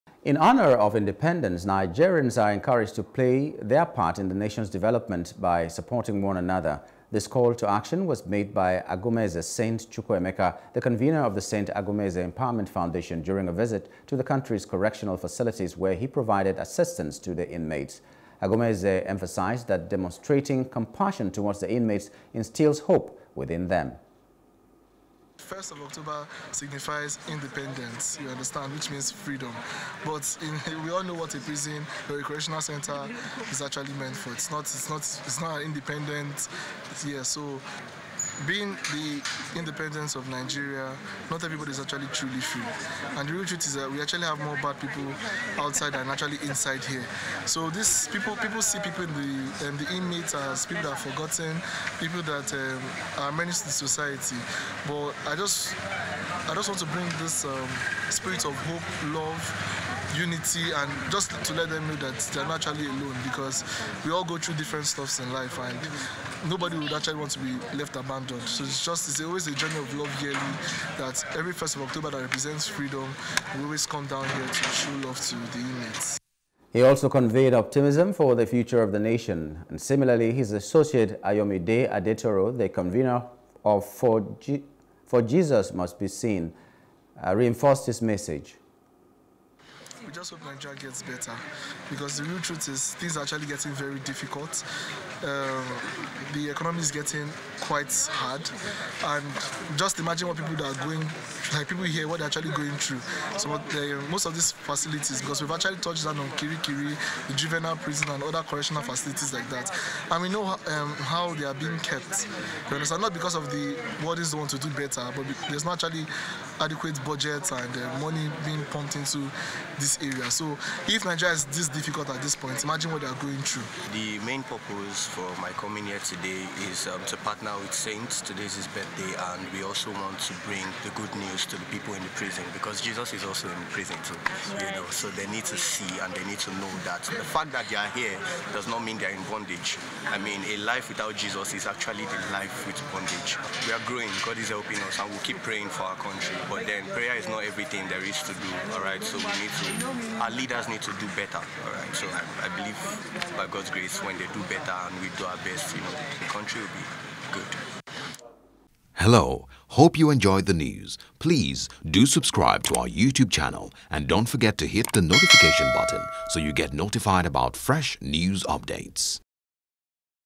Nigeria@64: Groups Give Support To Inmates On Independence Day
In honour of Independence, Nigerians are encouraged to play their part in the nation's development by supporting one another. This call to action was made by Agomeze Saint Chukwuemeka, the Convener of the Saint Agomeze Empowerment Foundation, during a visit to the country's correctional facilities where he provided assistance to the inmates.
Agomeze emphasized that demonstrating compassion towards the inmates instils hope within them.
He also conveyed optimism for the future of the nation. Similarly, his associate, Ayomide Adetoro, the Convener of For Jesus Must Be Seen, reinforced this message.